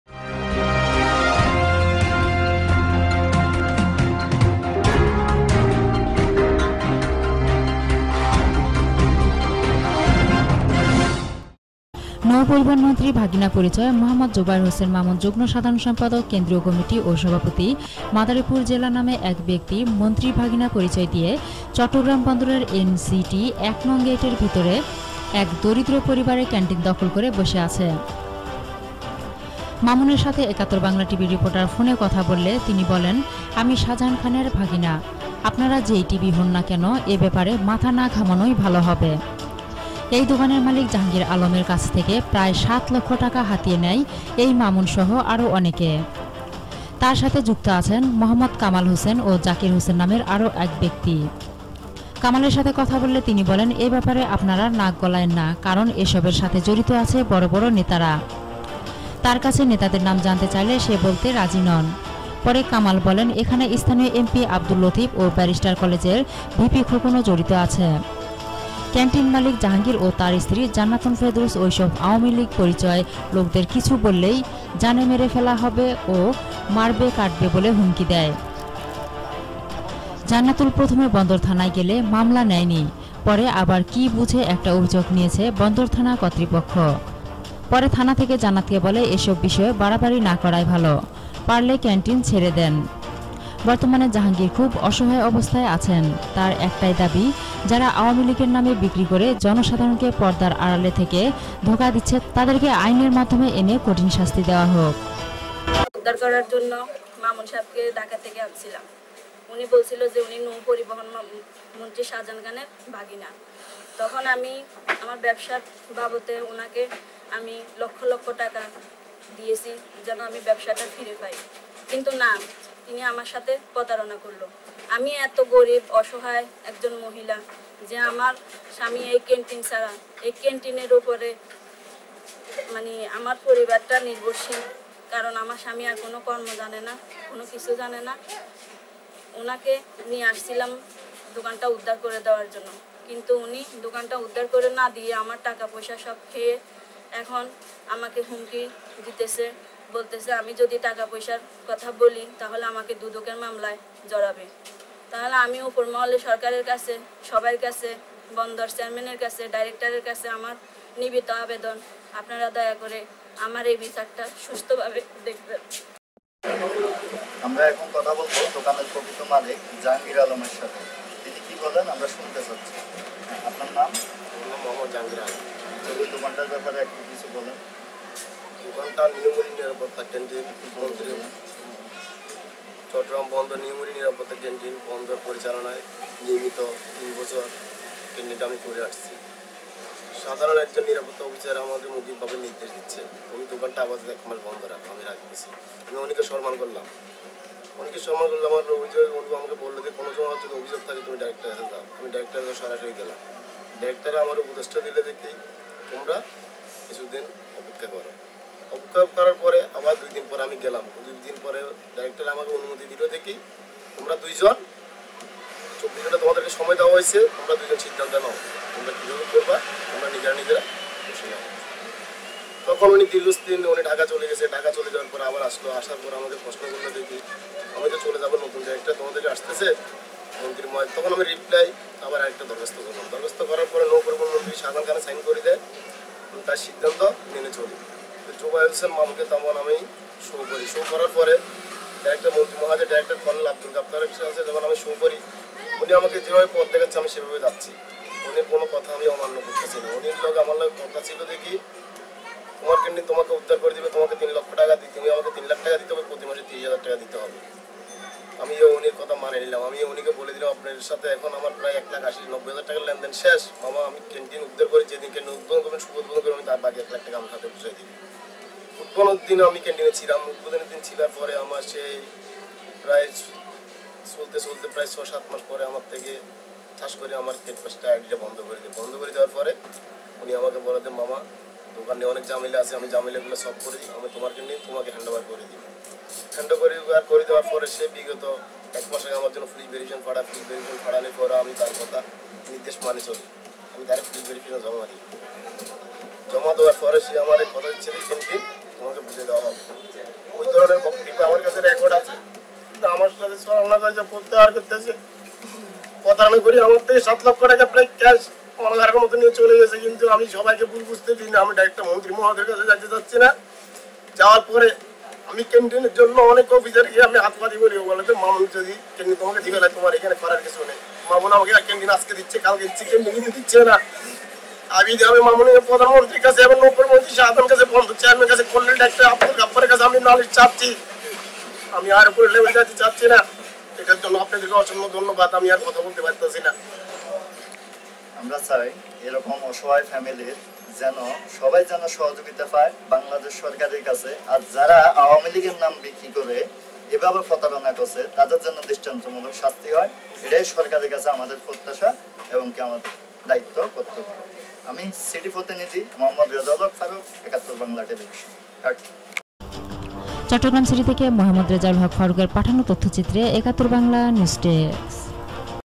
নৌ-পরিবহন মন্ত্রীর ভাগিনা পরিচয়ে বন্দরে ক্যান্টিন দখল|CTG city|71Bangla TV
নৌ-পরিবহন মন্ত্রীর ভাগিনা পরিচয়ে মোঃ জুবায়ের হোসেন মামুন যুগ্ন সাধারণ সম্পাদক কেন্দ্রীয় কমিটি ও সভাপতি, মাদারীপুর জেলা  নামে এক ব্যাক্তি মন্ত্রীর ভাগিনা পরিচয় দিয়ে চট্টগ্রাম বন্দরের এন.সি.টি ১নং গেইটের ভিতরে এক দরিদ্র পরিবারের ক্যান্টিন দখল করে বসে আছে। মামুনের সাথে ৭১ বাংলা টিভির রিপোর্টার ফোনে কথা বললে তিনি বলেন আমি শাহজান খানের ভাগিনা আপনারা যেই টিভি হোন না কেন এ ব্যাপারে মাথা না ঘামানোই ভালো হবে। এই দোকানের মালিক জাহাঙ্গীর আলম এর কাছ থেকে প্রায় ৭,০০,০০০/=লক্ষ টাকা হাতিয়ে নেয় এই মামুন সহ আরো অনেকে। তার সাথে যুক্ত আছেন মোঃ কামাল হোসেন ও জাকির হোসেন নামের আরো ব্যাক্তি। কামালের সাথে কথা বললে তিনি বলেন এ ব্যাপারে আপনারা নাক গলাইয়েন না কারন এ সবের সাথে জড়িত আছে বড় বড় নেতারা। তার কাছে নেতাদের নাম জানতে চাইলে সে বলতে রাজি নন। পরে কামাল বলেন এখানে স্থানীয় এম.পি আব্দুল লতিফ ও ব্যারিস্টার কলেজের ভিপি খোকনও জড়িত আছে। ক্যান্টিন মালিক জাহাঙ্গীর ও তার স্ত্রী জান্নাতুল ফেরদাউস ঐ সব আওয়ামী পরিচয়ের লোকদের কিছু বললেই জানে মেরে ফেলা ও মারবে কাটবে বলে হুমকি দেয়। জান্নাতুল প্রথমে বন্দর থানায় গেলে মামলা নেয়নি, পরে আবার কি বুঝে একটা অভিযোগ নিয়েছে বন্দর থানা কর্তৃপক্ষ। তারপর থানার থেকে জান্নাতকে বলে এসব বিষয়ে বাড়াবাড়ি না করাই ভালো। পারলে ক্যান্টিন ছেড়ে দেন। বর্তমানে জাহাঙ্গীর খুব অসহায় অবস্থায় আছেন। তার একটাই দাবী যারা আওয়ামী-লীগের নাম বিক্রি করে জনসাধারণকে পর্দার আড়ালে থেকে ধোকা দিচ্ছে তাদেরকে আইনের মাধ্যমে কঠিন শাস্তি দেওয়া হোক।